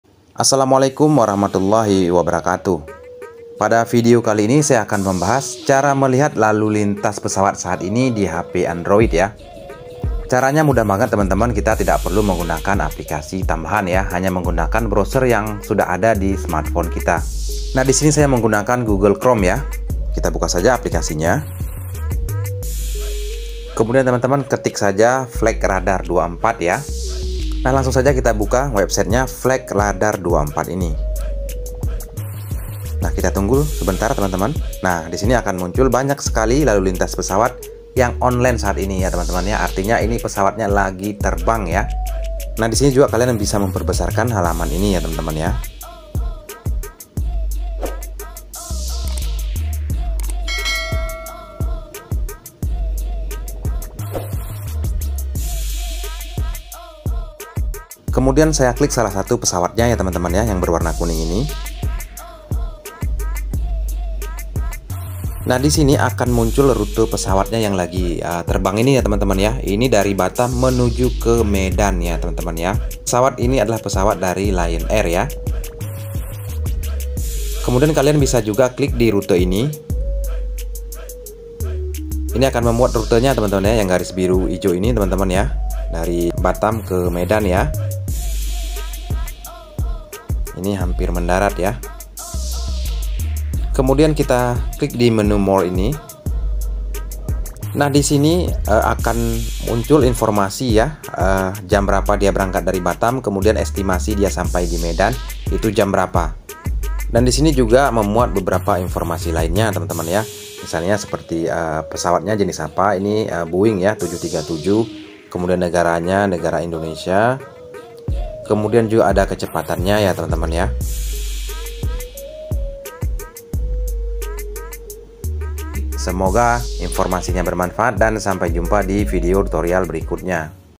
Cara Melihat Lalu Lintas Pesawat Di Hp Android
Pada video kali ini saya membahas bagaimana caranya melihat lalu lintas pesawat dan juga rutenya melalui hp android tanpa perlu mendownload aplikasi yang lainnya. Bagi yang belum tau caranya mari nonton video ini.